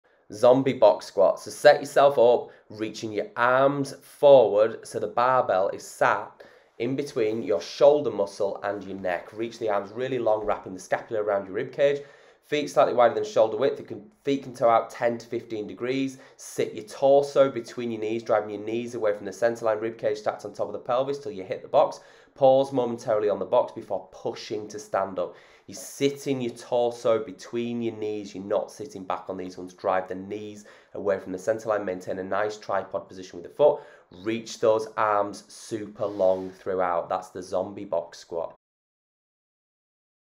Zombie Box Squat
Description:
Welcome to FitCureTV, your go-to source for fitness inspiration and guidance! In today's video, we're diving into an unconventional yet highly effective squat variation that will challenge your lower body strength and stability – the Zombie Squat.

💀 Zombie Squat:
Prepare to take your squat game to the next level with the Zombie Squat! This unique exercise engages your quads, hamstrings, glutes, and core while also improving your balance and coordination. Our experienced trainers will walk you through the correct form and execution, ensuring you reap the maximum benefits from this challenging movement.

With its emphasis on maintaining an upright torso and an extended arm position, the Zombie Squat encourages proper posture and functional strength – perfect for athletes, fitness enthusiasts, and anyone seeking a fresh challenge.

Join us as we break down the Zombie Squat step by step, offering valuable insights and modifications to accommodate various fitness levels.

👍 Ready to tackle the Zombie Squat? Give that like button a thumbs up!
🗣️ Have questions or feedback? Drop us a comment – we're here to help!

Thanks for tuning in – let's squat our way to stronger legs and a fitter lifestyle, together! 💪🏋️‍♀️